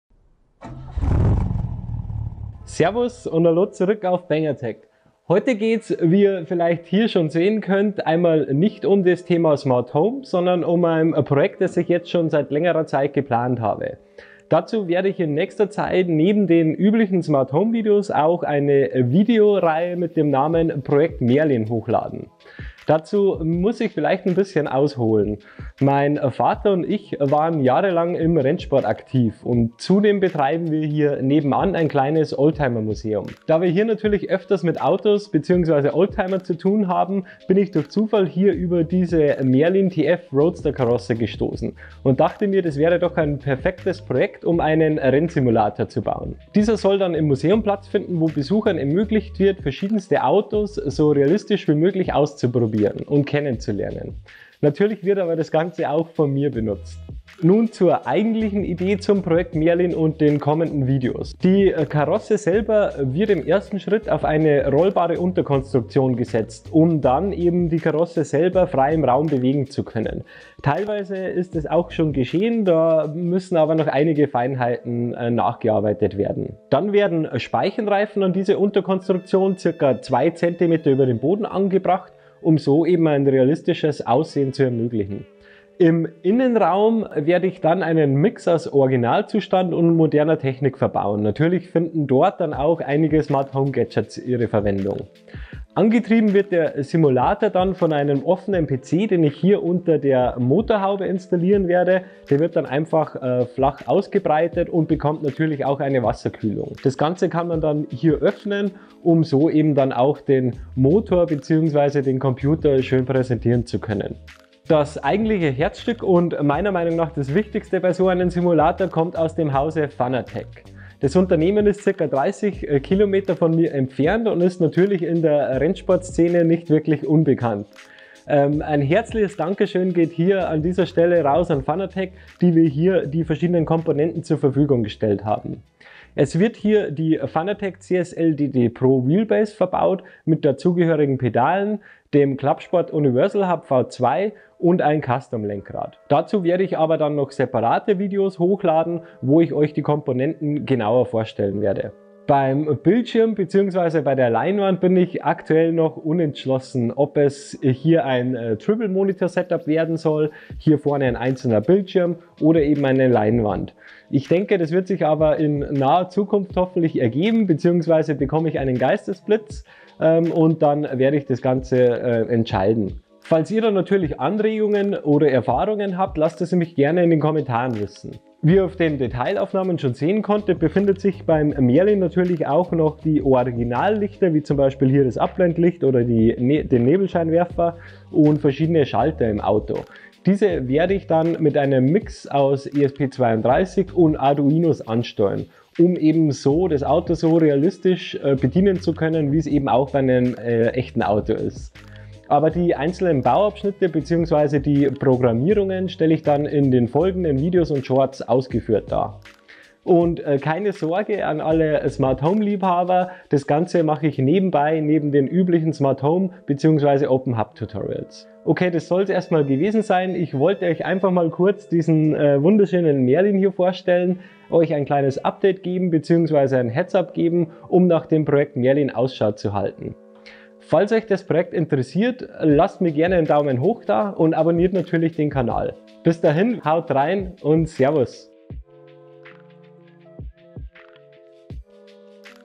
SIM RACING im OLDTIMER!? | PROJECT MERLIN | Verrückter RENNSIMULATOR | Sim Rig Konzept
Das ist der Startschuss für das Project Merlin.
In diesem Video stelle ich euch mein aktuelles Sim Racing Projekt vor. Dieses werde ich in mehrere Videos und Shorts aufteilen um euch bei dem spannenden Bau und Konstruktionsprozess mitzunehmen.

Vielen Dank an Fanatec für die Unterstützung!

My Gear:
⇢ Sonoff NS-Panel